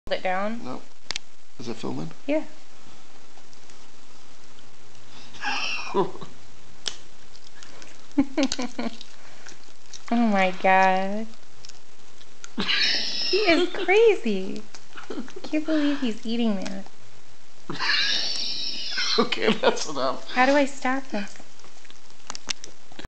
Lhasa Apso eating a candy cane
Our dog Buddy will eat anything you put in front of him.